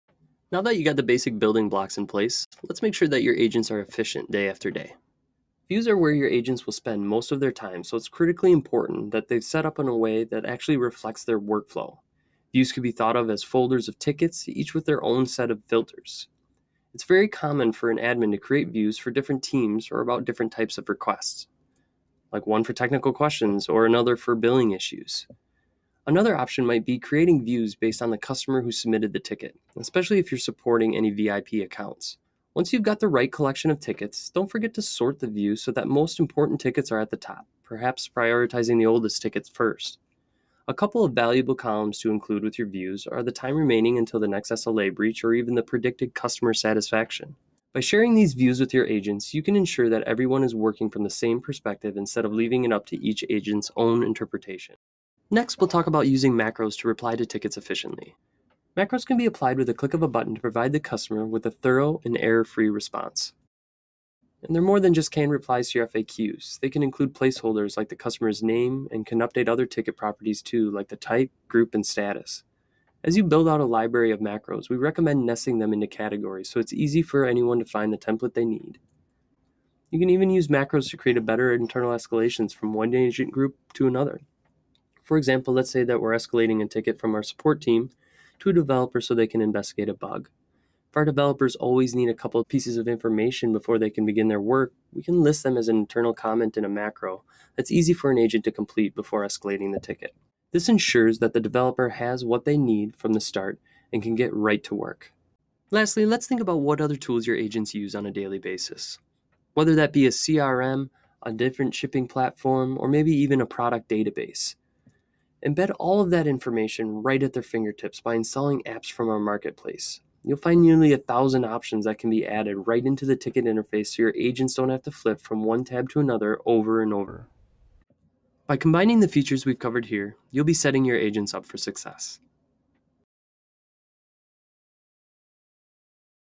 Zendesk Tutorial: Drive Agent Efficiency
Learn the basic ways you can use Zendesk to drive support agent efficiency.